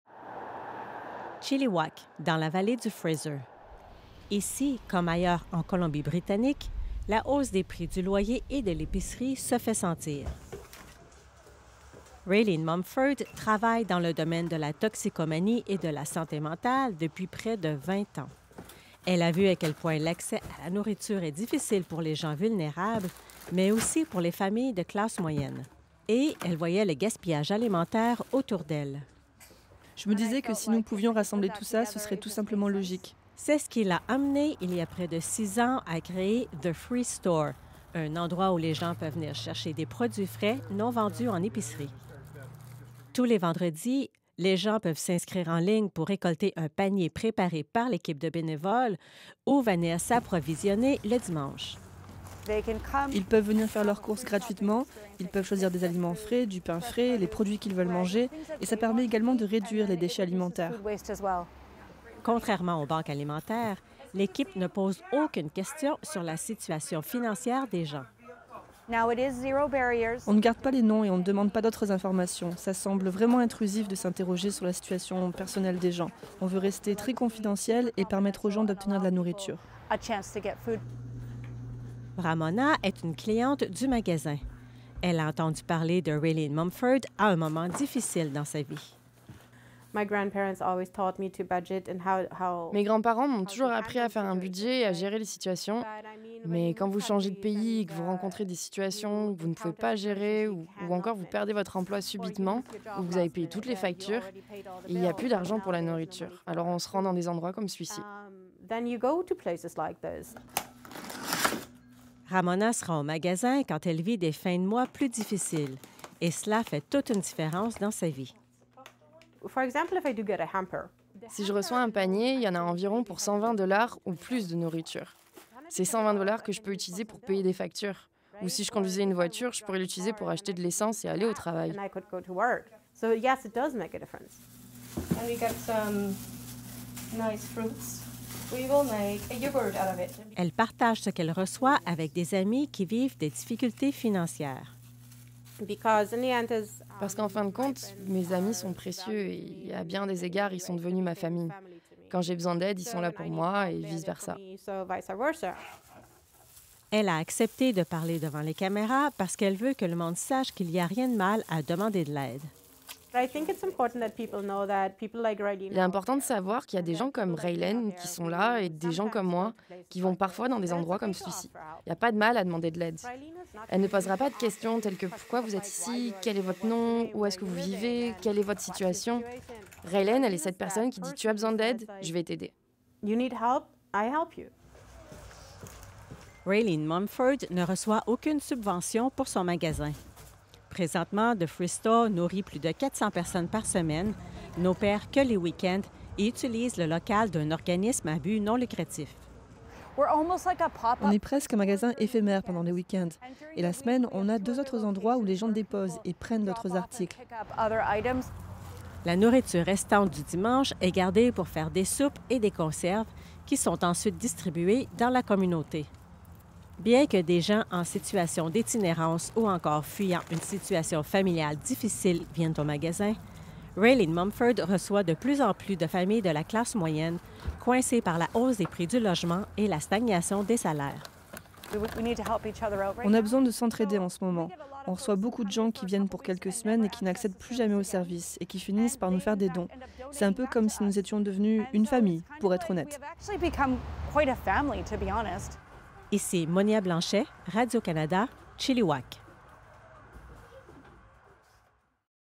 Le Free Store: des produits frais pour manger à sa faim en toute dignité
À Chilliwack, une initiative permet de distribuer de la nourriture à des familles dans le besoin.  The Free Store facilite l’accès à des denrées alimentaires et ce, dans la plus grande confidentialité et dignité.

Le reportage de Monia Blanchet.